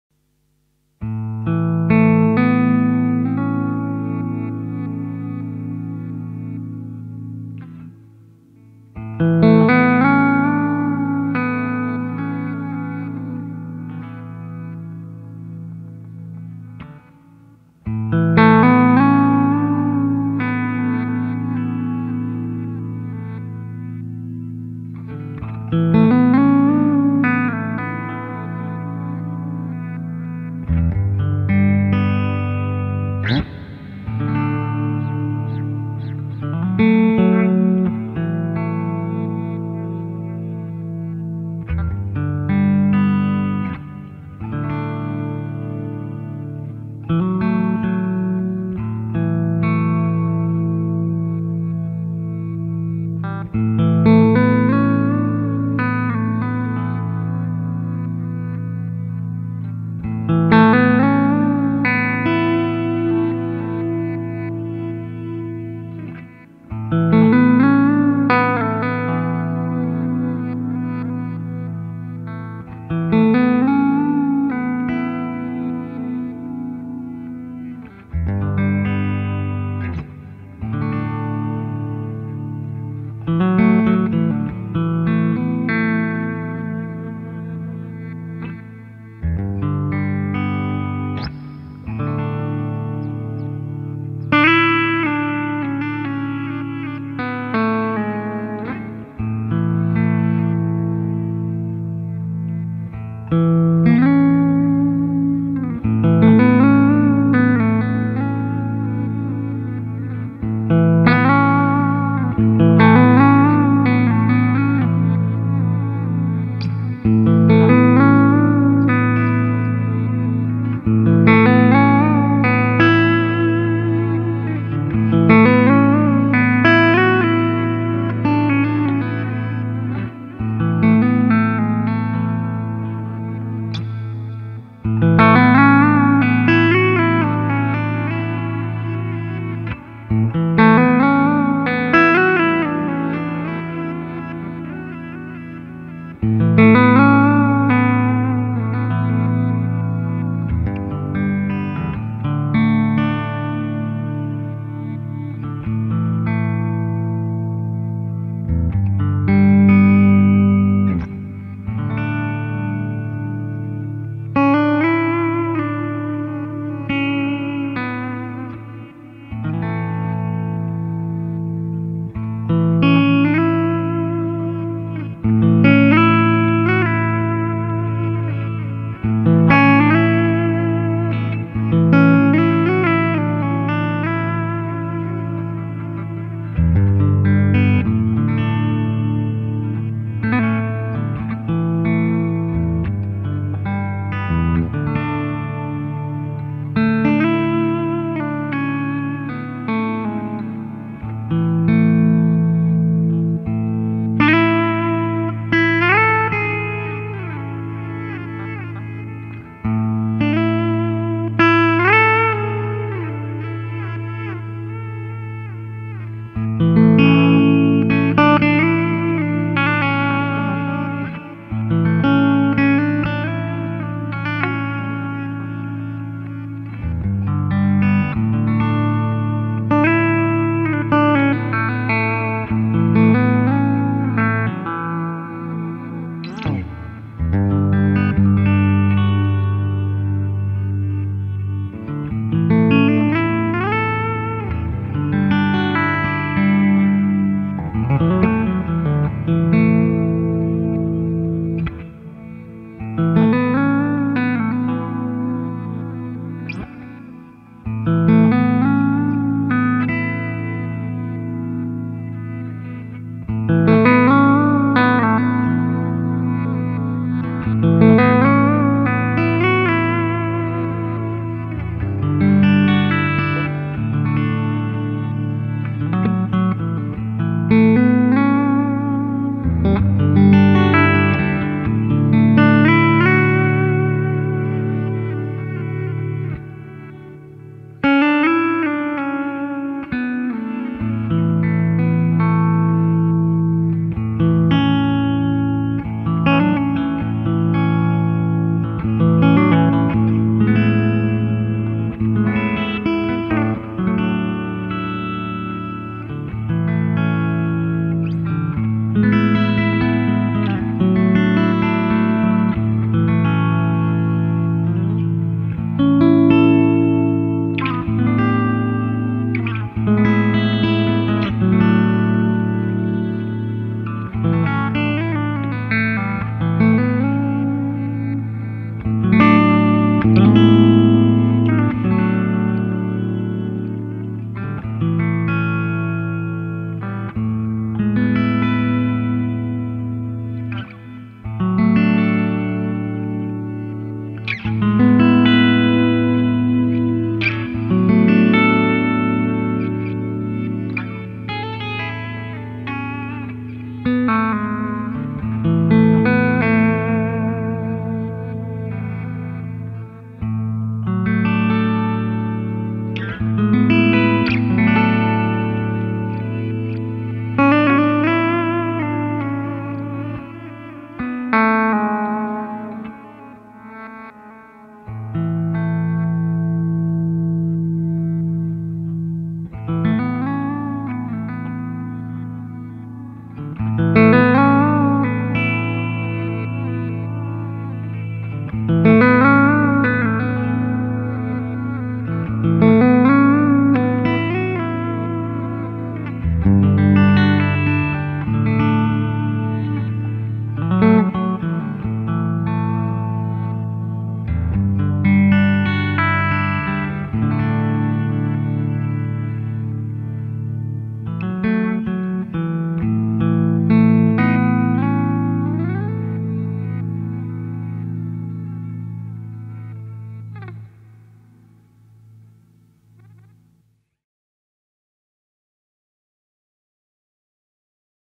Orange Guitar Meditation
Eine bluesige Meditation an der Gitarre zur Farbe Orange.
A Meditation on guitar related to the Color " Orange "